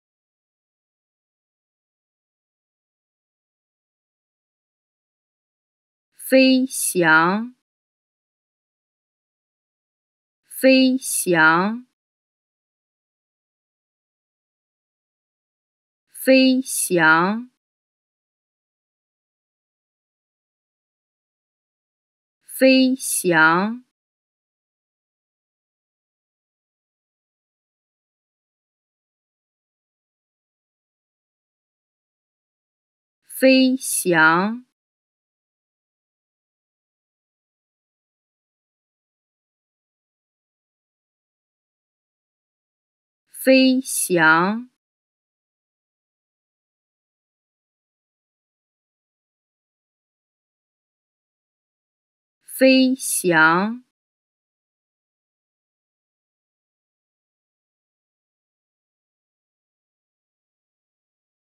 How To Say ''Fly'' (飞翔) in Mandarin Chinese
Learn how to pronounce ''Fly'' (飞翔) in Mandarin Chinese with this short pronunciation video